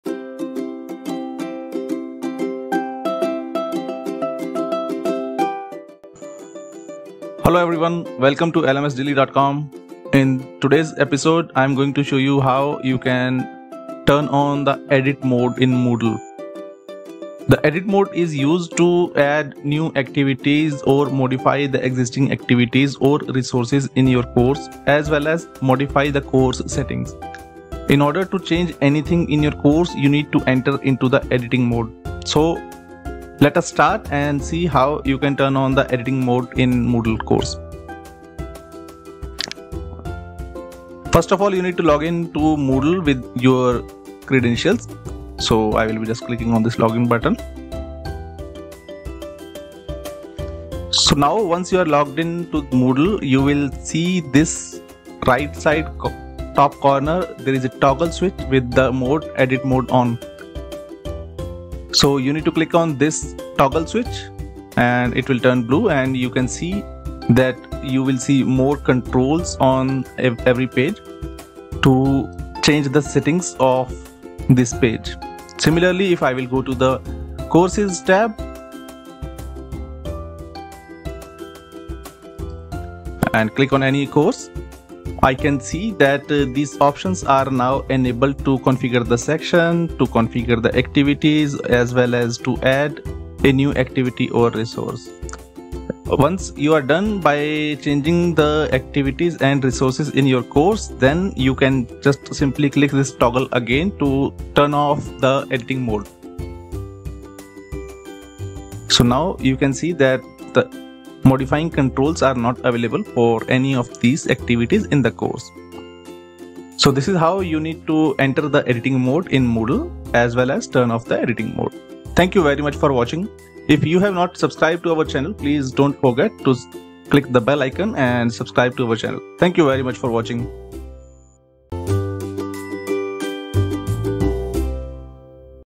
How to turn on the editing mode in Moodle
In this video, we will see how you can turn the Edit mode on in Moodle. The Turn Edit On or Off option can be found as a link in top right corner or as a toggle switch in recent Moodle versions.  This option is only available to teachers of the course who also have editing rights or to those users who are assigned site administration rights. Turn editing on allows teachers to change the appearance and functionality of the course. You can turn editing off again by pressing the button or the admin block link again.